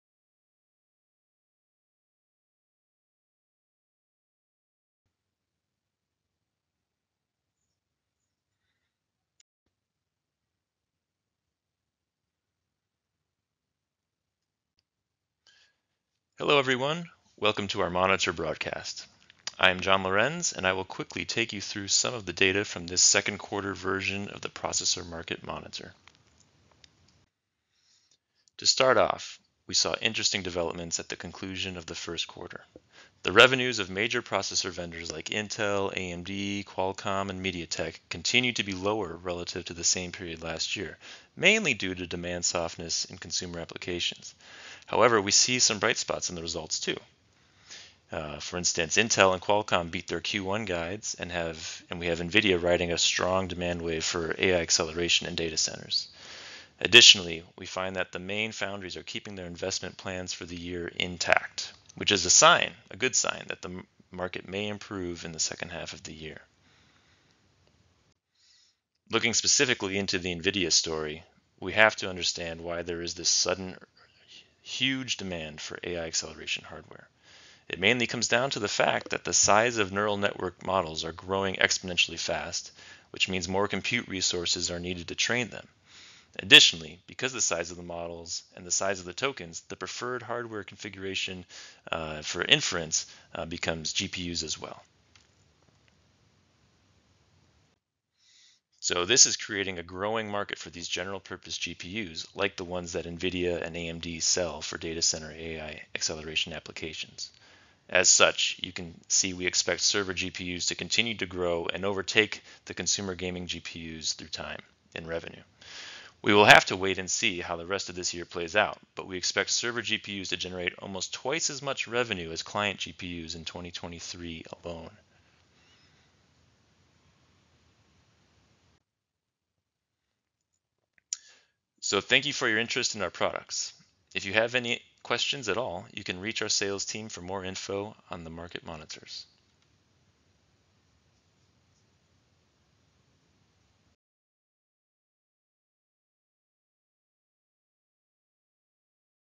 Yole Intelligence - Processor Market Monitor Q2 2023 - Broadcast
Plummeting consumer demand has weakened the CPU and GPU markets,  as AI innovation and chiplet integration continue to advance.